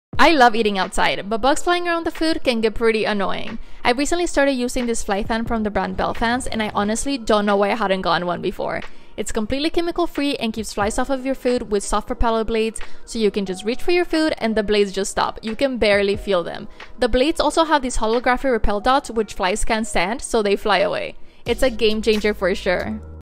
Bye Bye Flies: Keeping Your Food Fly-Free with Our Repellent Fan!
Are you tired of dealing with pesky flies ruining your outdoor meals? Say no more! In this exciting reel video, we introduce the ultimate fly repellent fan that will revolutionize your dining experience. No more shooing away flies or worrying about food contamination.